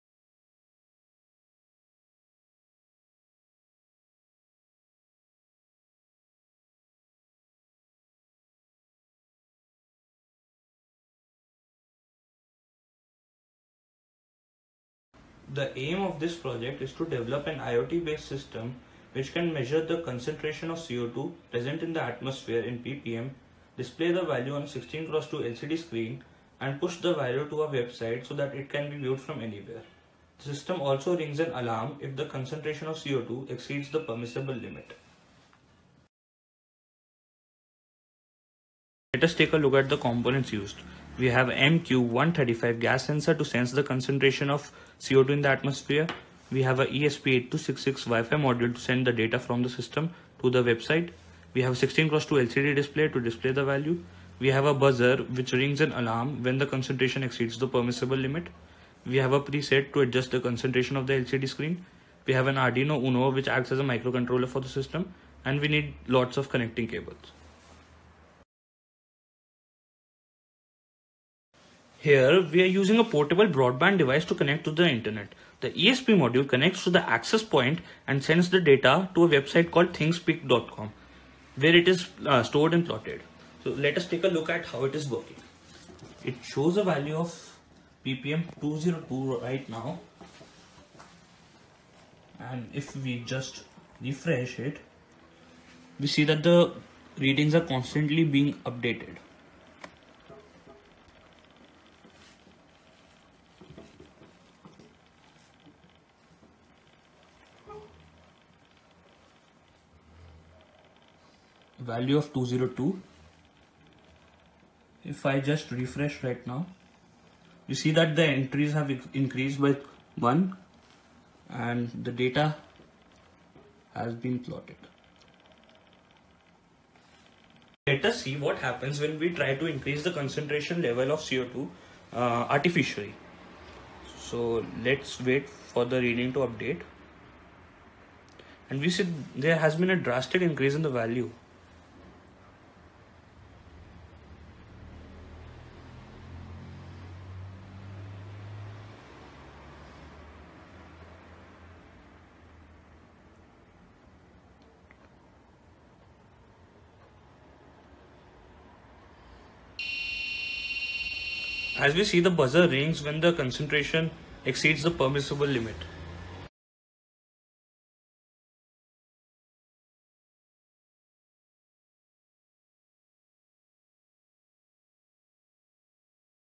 IoT based Air Pollution Monitoring System
The aim of this project is to develop an IoT based system which measures the amount of Co2 in  the atmosphere (in ppm) and pushes the data to thingspeak.com where it is stored and plotted. The arduino uno communicates with the esp8266 via AT commands.